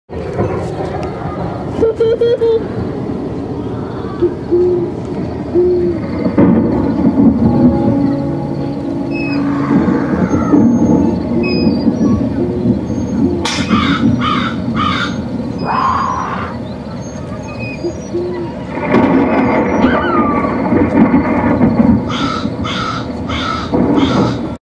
Tricycle In Action
Buckaneerbabe had a great idea for using our "ghostly footprints" with her little tricycle prop.  She wanted a child's giggle when the footprints started and then led to the tricycle.  When the footprints paused, the tricycle was lit and the skeleton kid on it starts pedaling.